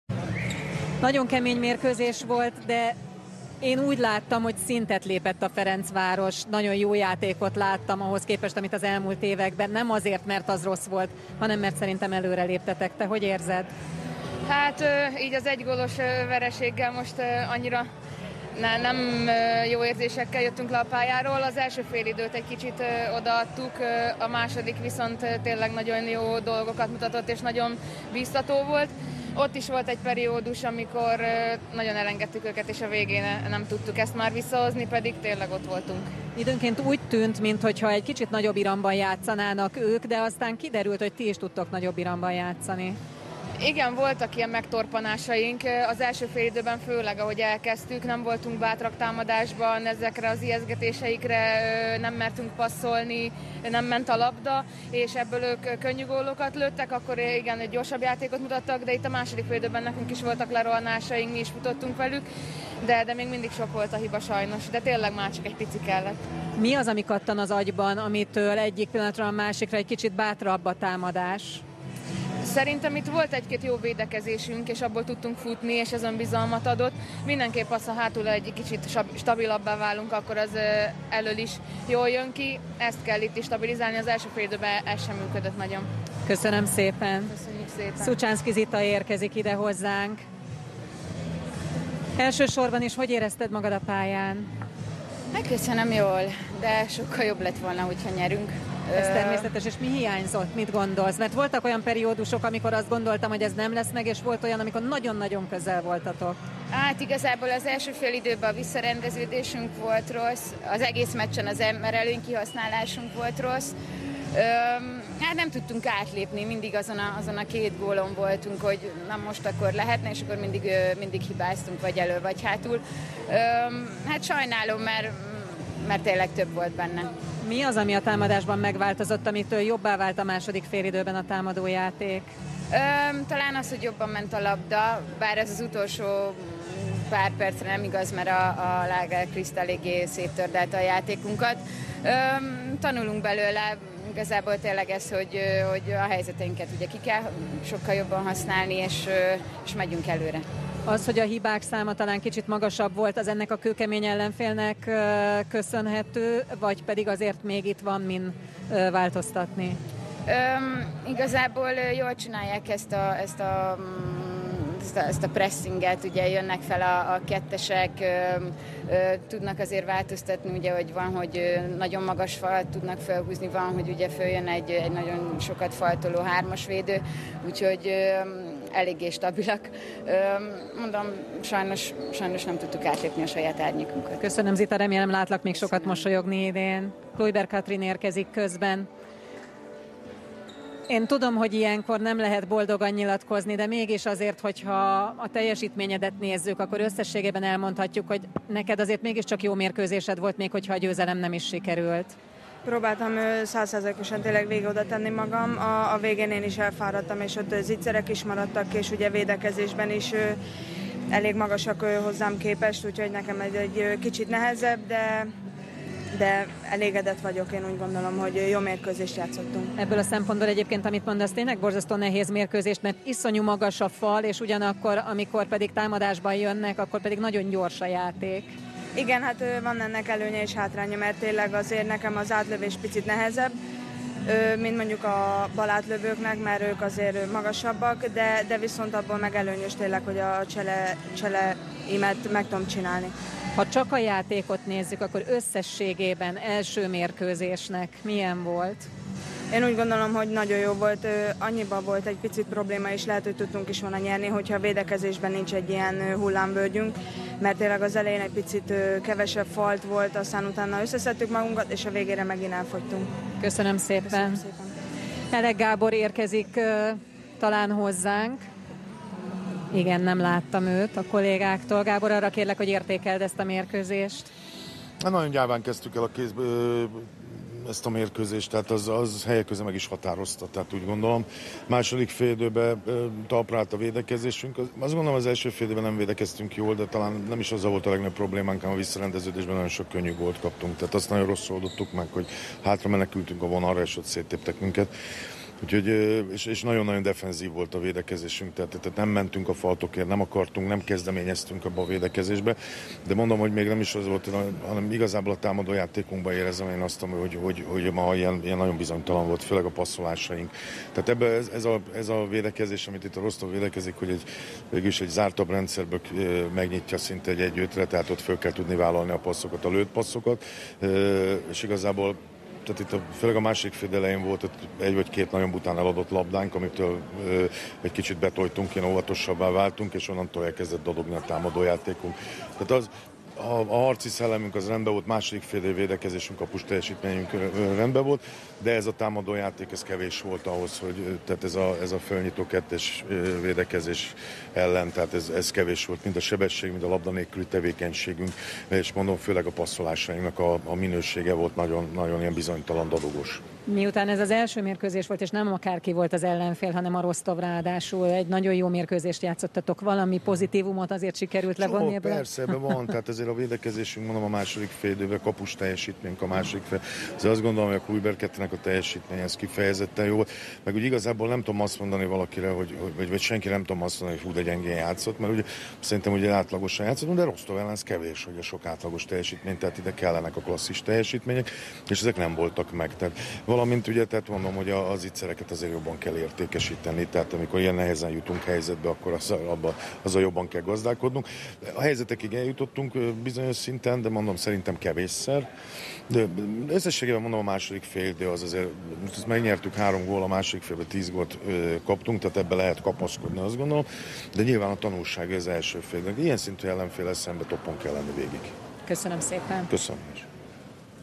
SportTV - Értékelések a Rosztov Don elleni vereség után
A Ferencvárosi Torna Club női kézilabda csapatának Bajnokok Ligája csoportkörös meccse után értékelt Klujber Katrin, Kovacsics Anikó, Szucsánszki Zita és Elek Gábor.